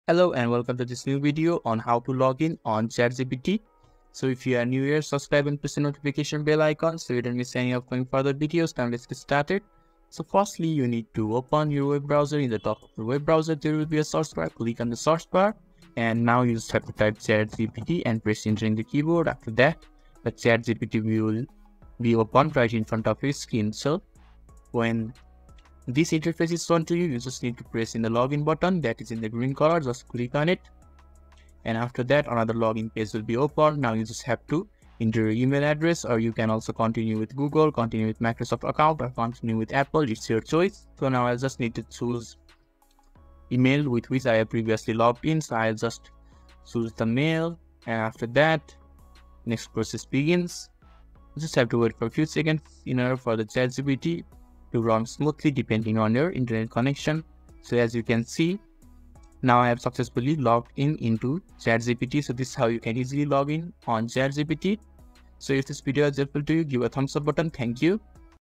How To Login To ChatGPT Account | ChatGPT Sign In 2024
Learn how to sign into your chatgpt account. So make sure to watch the video till the end and follow the stepa for easy login to chatgpt.